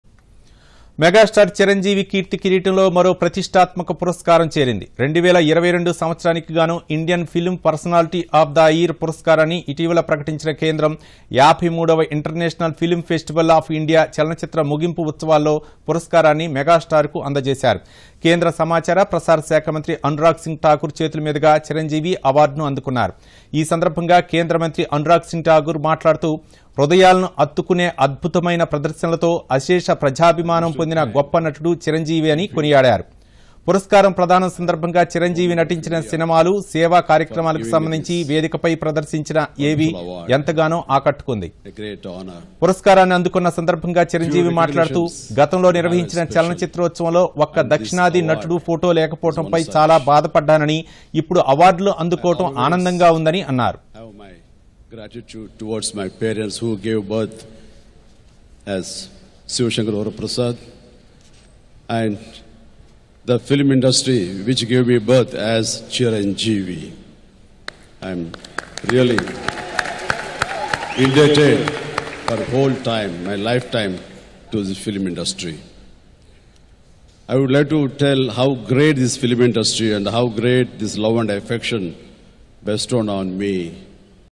Chiranjeevi Received the Indian Best Film Personality Award | 53rd IFFI Awards | Goa
మెగాస్టార్ చిరంజీవి కీర్తి కిరీటంలో మరో ప్రతిష్ఠాత్మక పురస్కారం చేరింది. 2022 సంవత్సరానికిగాను ఇండియన్ ఫిల్మ్ పర్సనాలిటీ ఆఫ్ ద ఇయర్ పురస్కారాన్ని ఇటీవలే ప్రకటించిన కేంద్రం 53వ ఇంటర్నేషనల్ ఫిల్మ్ ఫెస్టివల్ ఆఫ్ ఇండియా చలనచిత్ర ముగింపు ఉత్సవాల్లో పురస్కారాన్ని మెగాస్టార్ కు అందజేశారు. కేంద్ర సమాచార, ప్రసార శాఖ మంత్రి అనురాగ్ సింగ్ ఠాకూర్ చేతుల మీదుగా చిరంజీవి అవార్డును అందుకున్నారు. ఈ సందర్భంగా కేంద్ర మంత్రి అనురాగ్ సింగ్ ఠాకూర్ మాట్లాడుతూ.. హృదయాలను హత్తుకునే అద్భుతమైన ప్రదర్శనలతో అశేష ప్రజాభిమానం పొందిన గొప్ప నటుడు చిరంజీవి అని కొనియాడారు. పురస్కారం ప్రదానం సందర్భంగా చిరంజీవి నటించిన సినిమాలు, సేవా కార్యక్రమాలకు సంబంధించి వేదికపై ప్రదర్శించిన ఏవీ ఎంతగానో ఆకట్టుకుంది. పురస్కారాన్ని అందుకున్న సందర్భంగా చిరంజీవి మాట్లాడుతూ గతంలో నిర్వహించిన చలన చిత్రోత్సవంలో ఒక్క దక్షిణాది నటుడి ఫొటో లేకపోవడంపై చాలా బాధపడ్డానని, ఇప్పుడు అవార్డుల అందుకోవడం ఆనందంగా ఉందని అన్నారు.